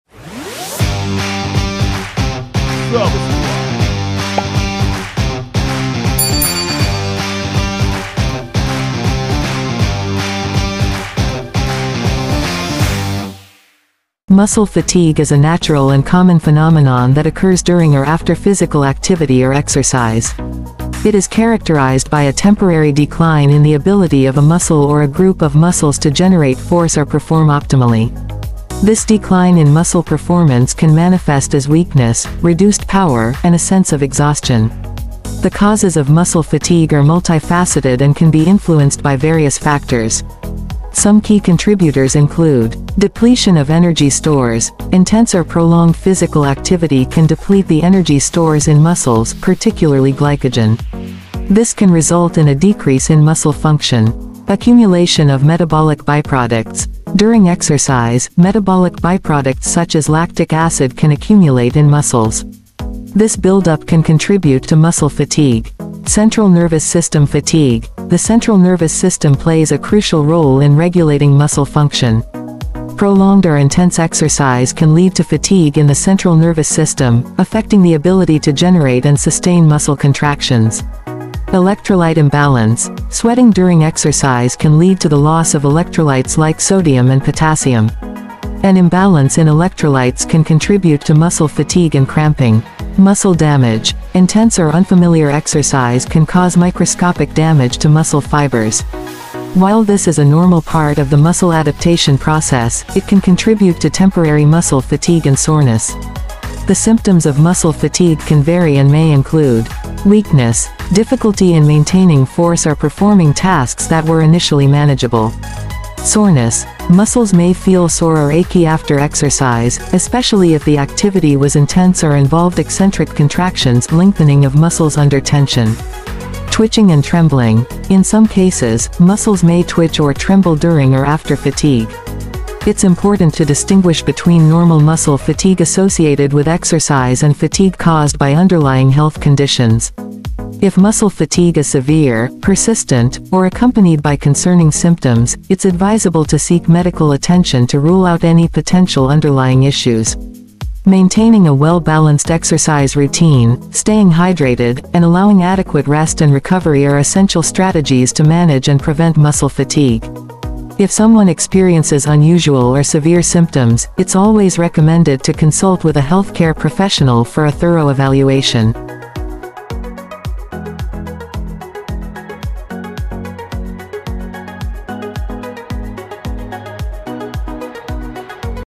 Understanding and Managing Muscle Fatigue During Exercise
Title: Understanding and Managing Muscle Fatigue During Exercise

Description: In this YouTube video, we will explore the topic of muscle fatigue, a common occurrence during or after physical activity or exercise. We will delve into the causes of muscle fatigue, including energy depletion, metabolic byproduct accumulation, central nervous system fatigue, electrolyte imbalances, and muscle damage. We will discuss the symptoms of muscle fatigue, such as weakness, soreness, twitching, and trembling. Additionally, we will emphasize the importance of distinguishing between normal muscle fatigue and fatigue caused by underlying health conditions. Tips on maintaining a well-balanced exercise routine, staying hydrated, and allowing adequate rest and recovery will also be shared. Join us to gain a better understanding of muscle fatigue and learn strategies to manage and prevent it effectively.